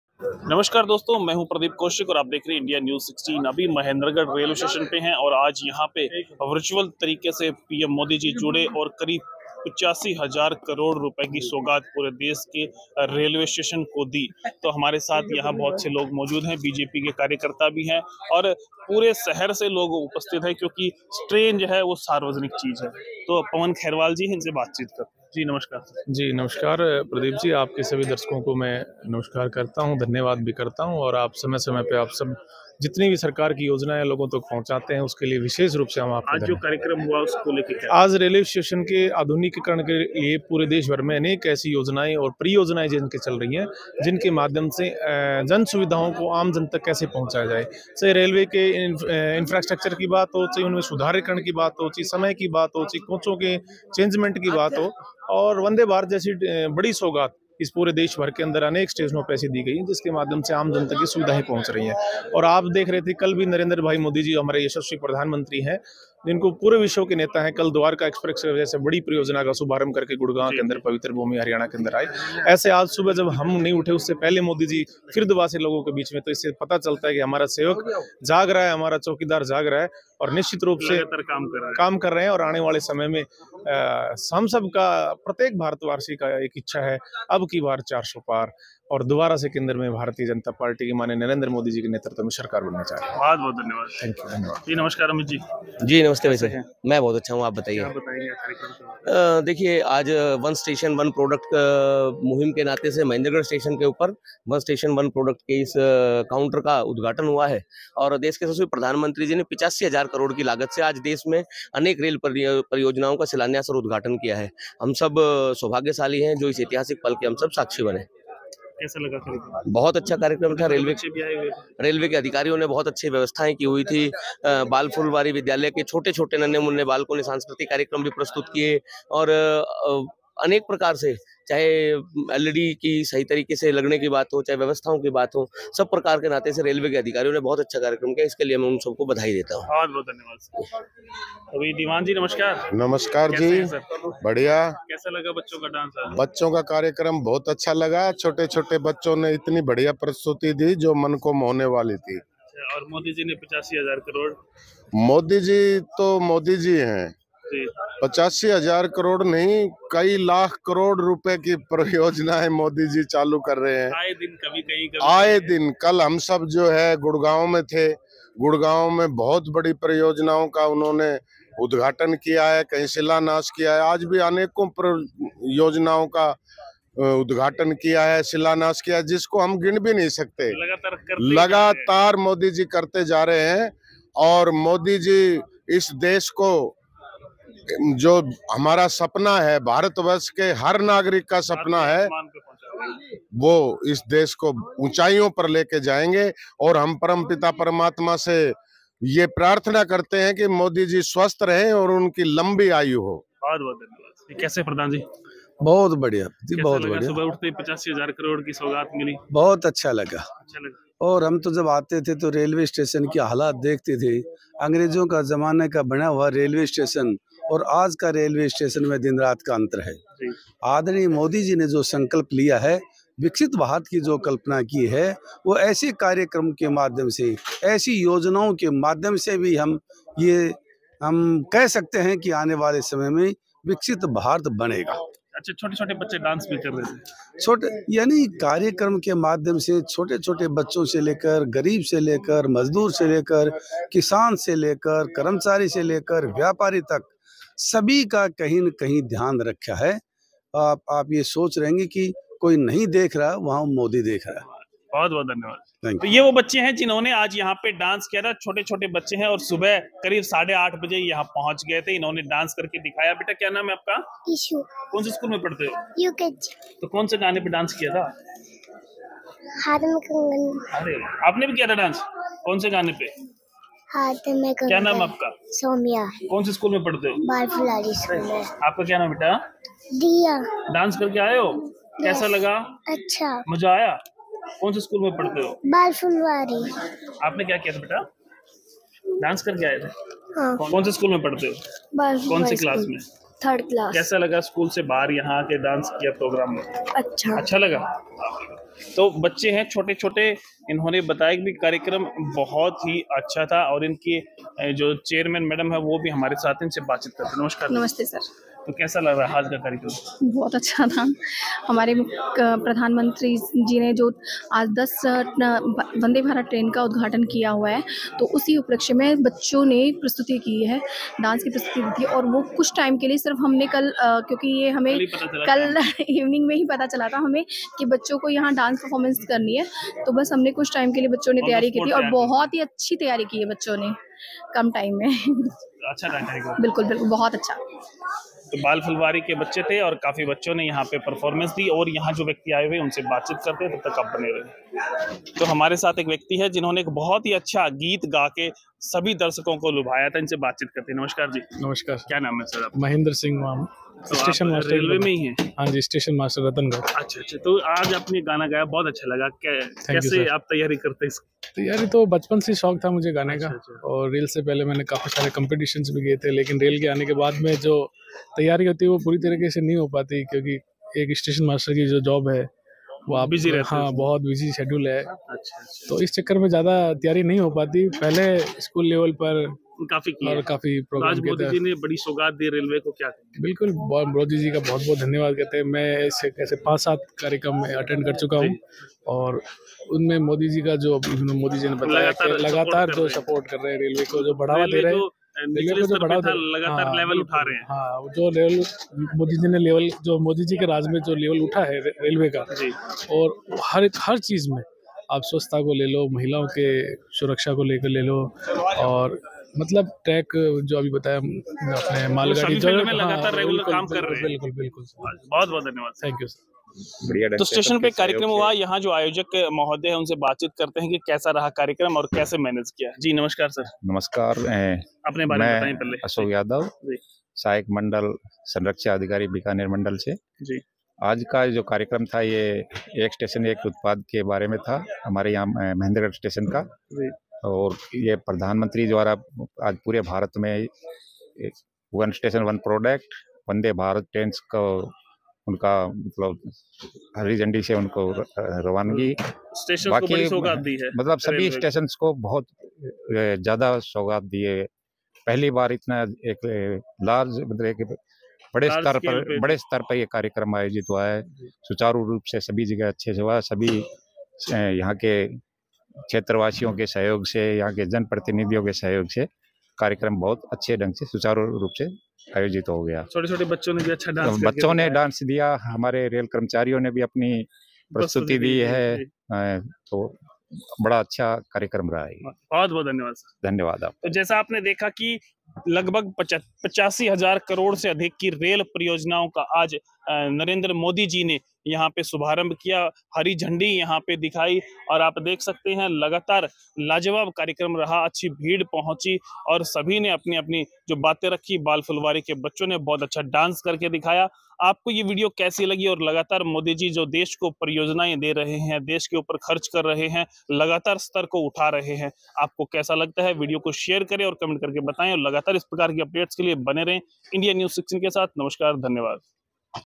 |85 हजार करोड़| PM Modi कार्यक्रम Railway Station Mahendergarh बच्चों ने किया मनमोहक नृत्य
85 हजार करोड़ PM Modi कार्यक्रम Railway Station Mahendergarh बच्चों ने किया मनमोहक नृत्य